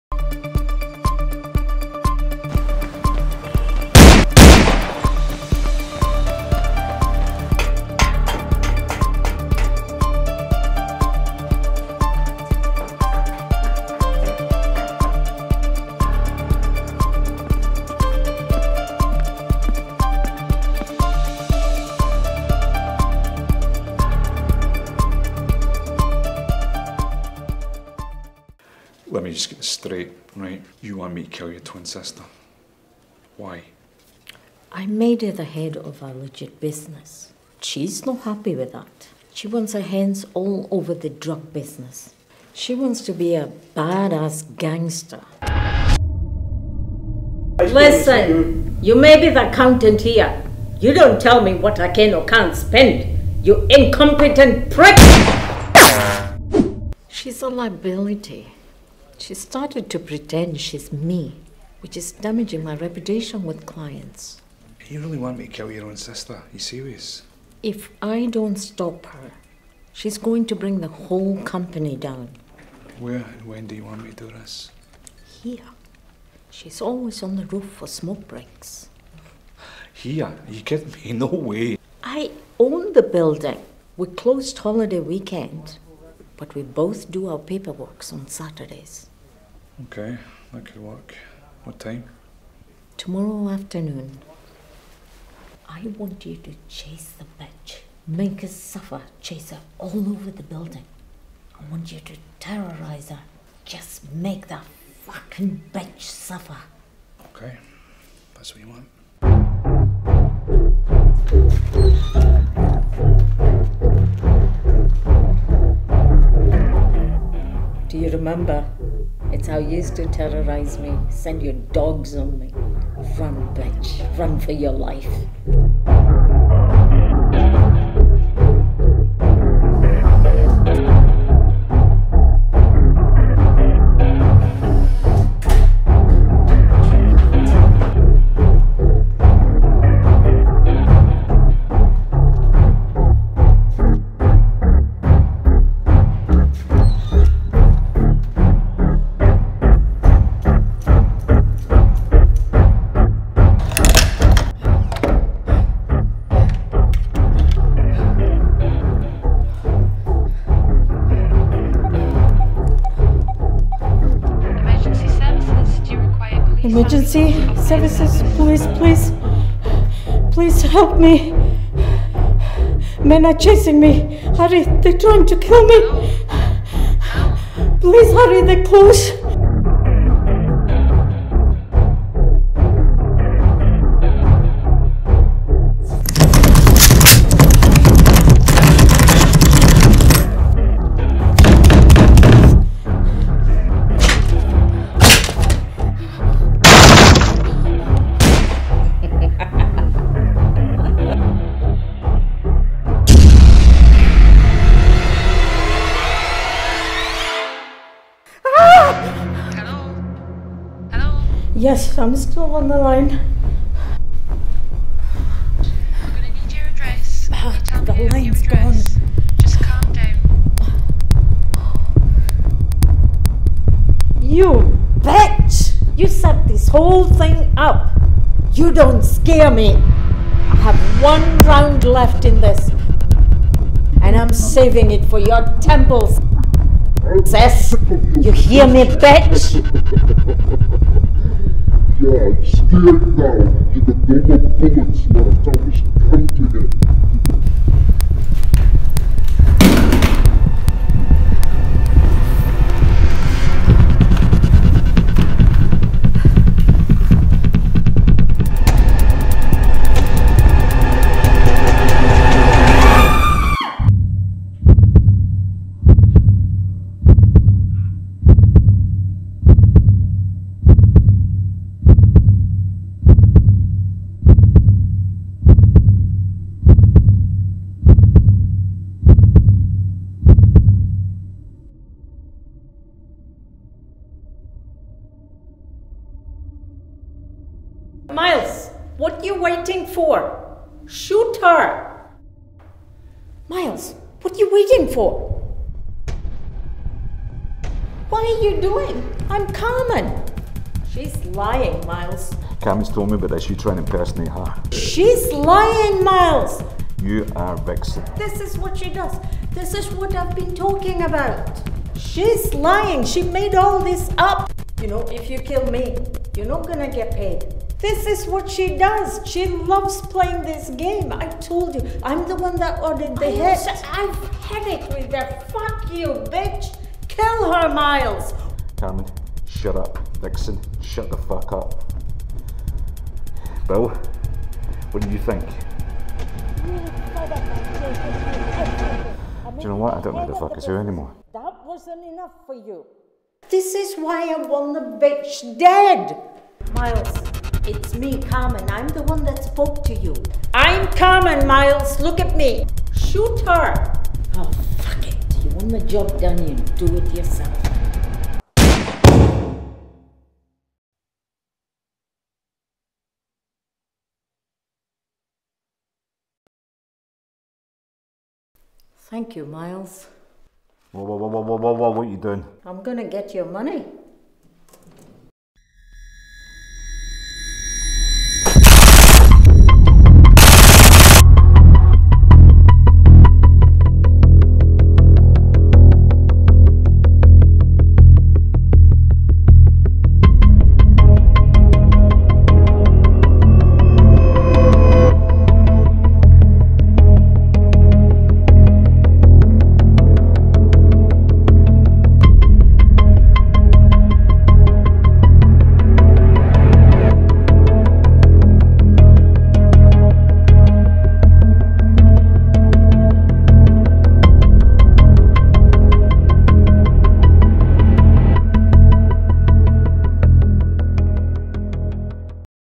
PSYCHO SISTERS - Model Hannah Kent -
The evil twins who are plotting to kill the other  which leads to double trouble